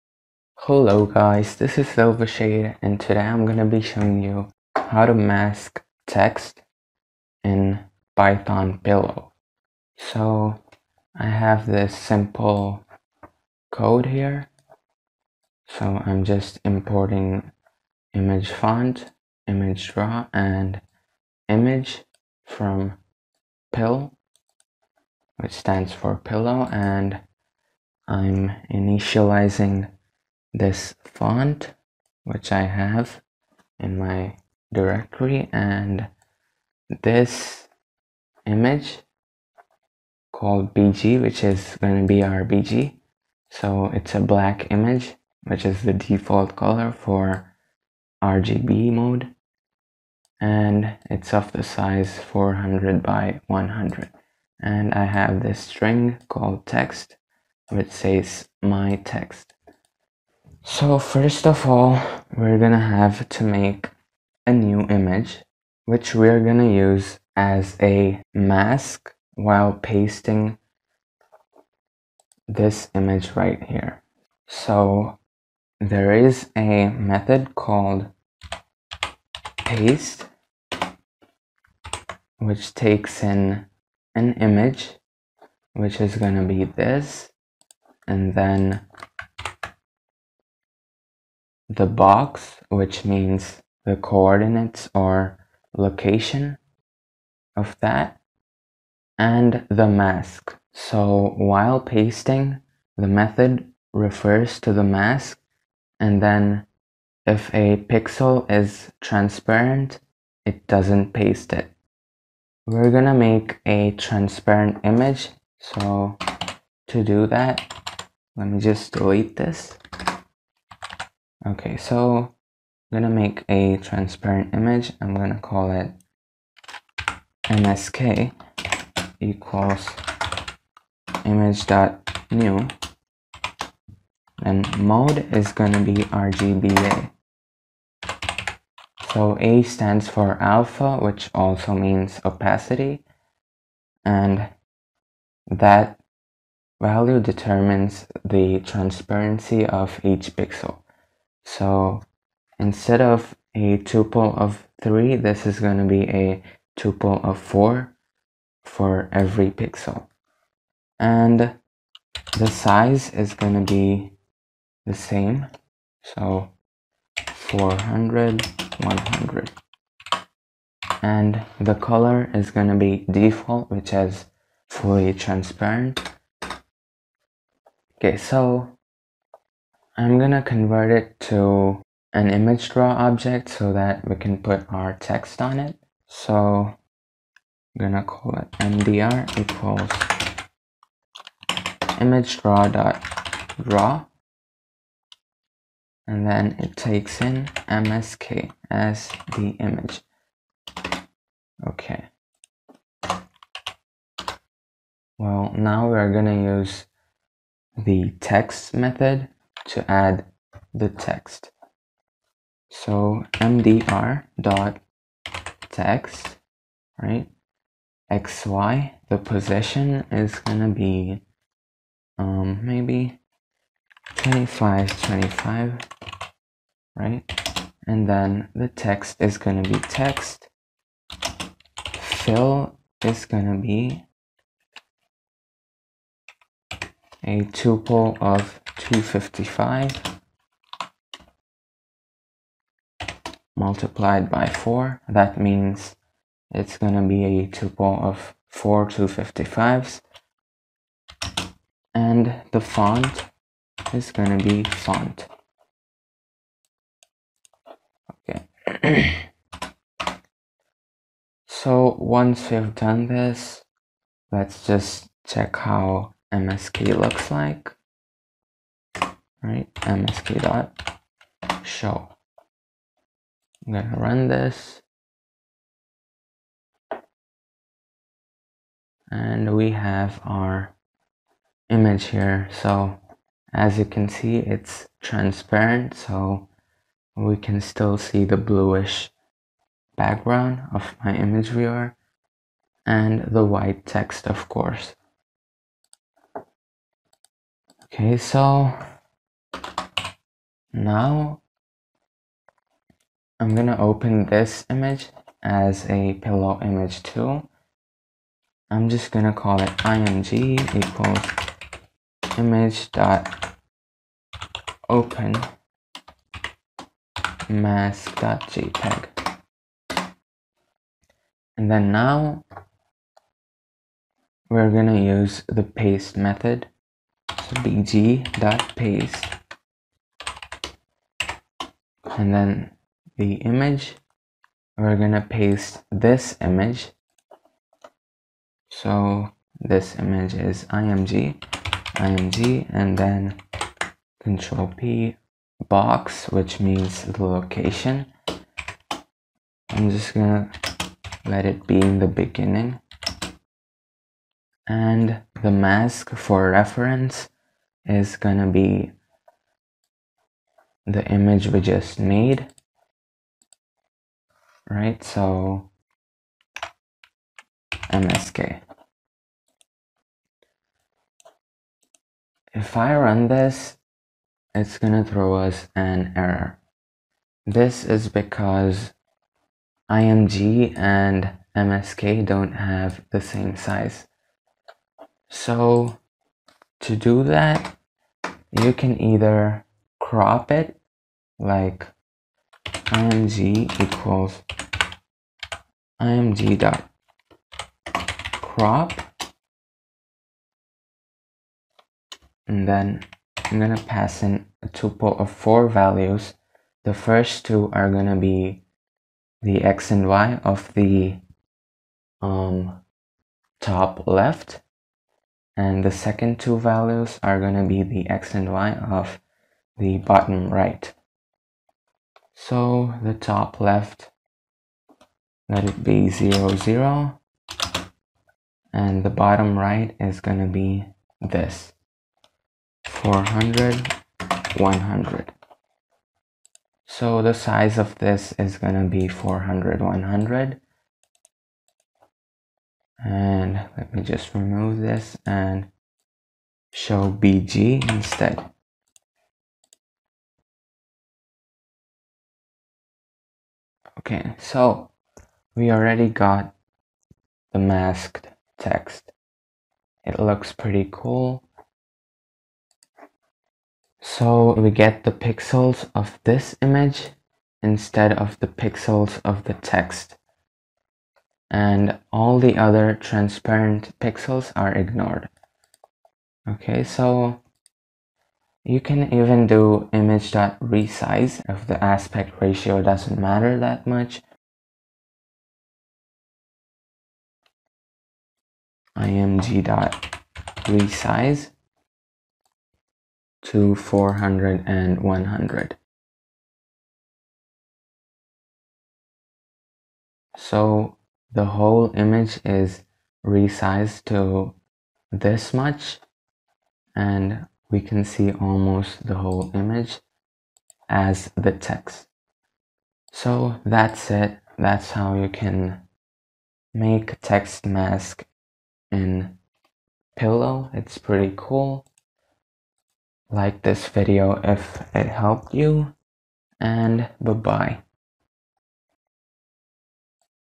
How to Create Text Mask in Pillow (PIL) - Python
In this video, I will be teaching you how to create a text mask (Image as text) using the Python Imaging Library.